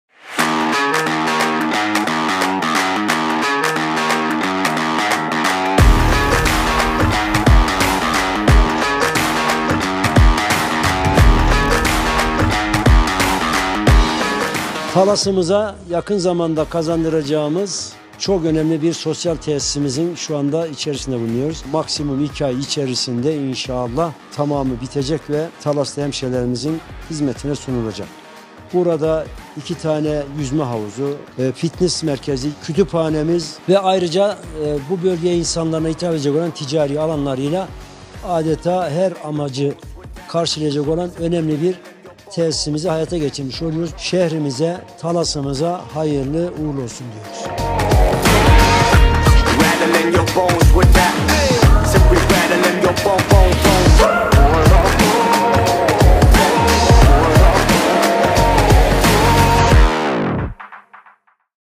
TALAS'A YÜZME HAVUZLU SOSYAL TESİS...
@kayseribuyuksehirbelediyes9898  Başkanı @baskanmemduhbyk ; Talas Mevlana Sosyal Yaşam Merkezi

Talas’ımıza çok amaçlı ve önemli bir tesis kazandırıyoruz.💪

🏊Yüzme Havuzları
🏋️Fitness Salonları
📚Kütüphane ve çok daha fazlası Sosyal Yaşam Merkezimizi yakın zamanda hayata geçiriyoruz.

Talas’ımıza ve şehrimize hayırlı olsun @talasbelediyesi9923 @mustafa7134. @QuantumTech @TALASFILM